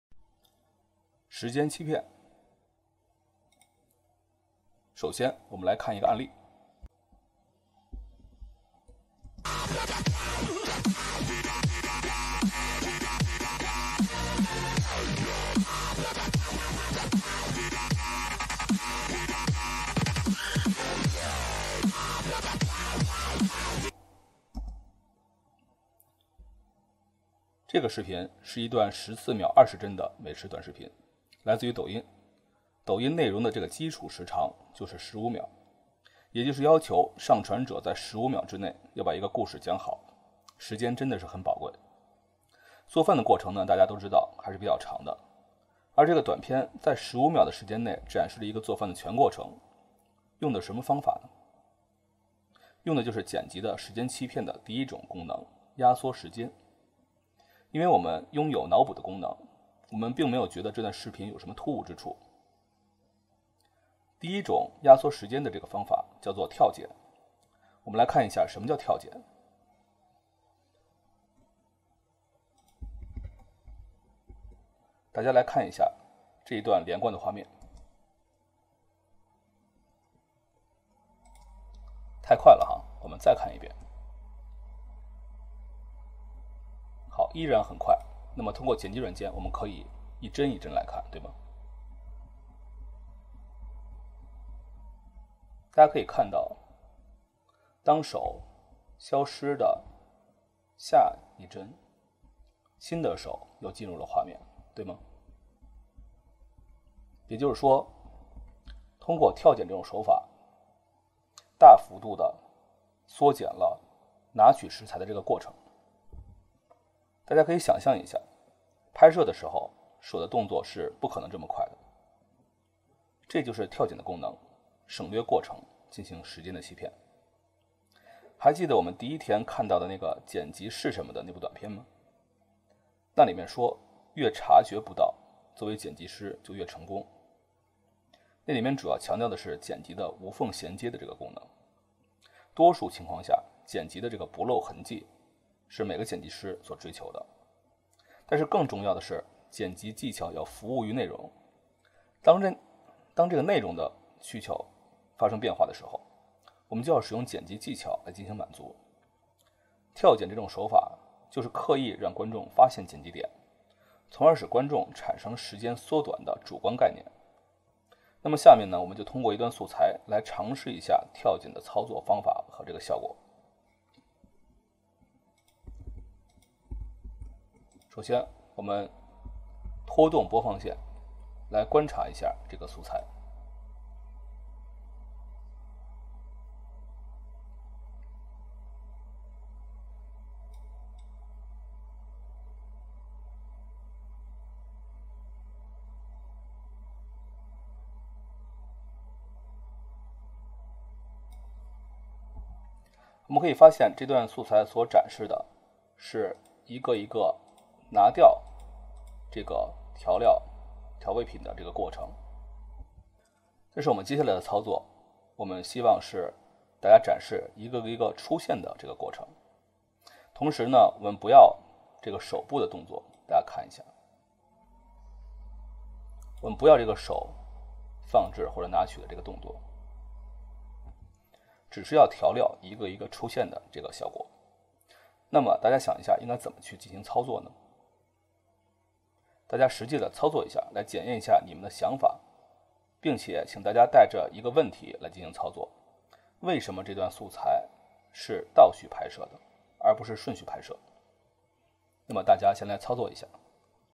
剪辑理念-时间欺骗 | 2.压缩时间
本课程教会你从现象看剪辑的本质并且应用于实际。